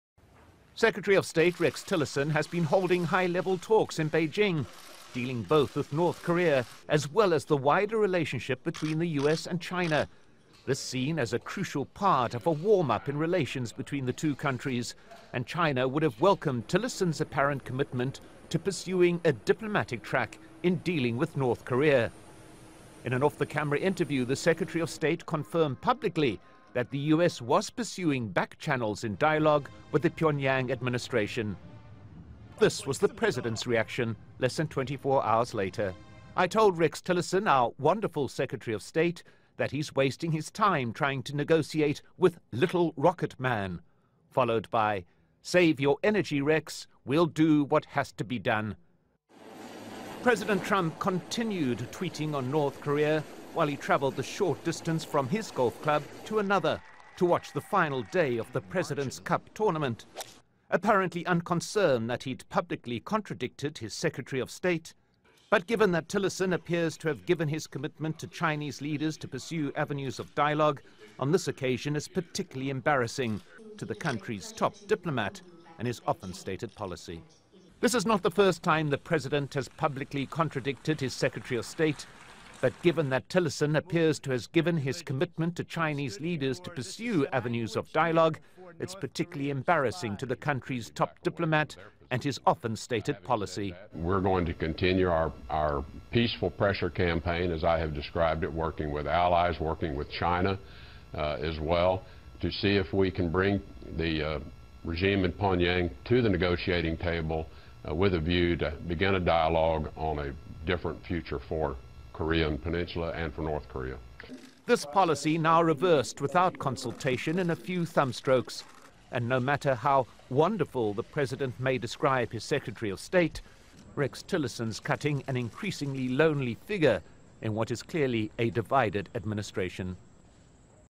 North Korea: Nuclear War May Break Out at 'Any Moment'
North Korea's deputy UN ambassador has warned the situation on the Korean Peninsula "has reached the touch-and-go point and a nuclear war may break out any moment".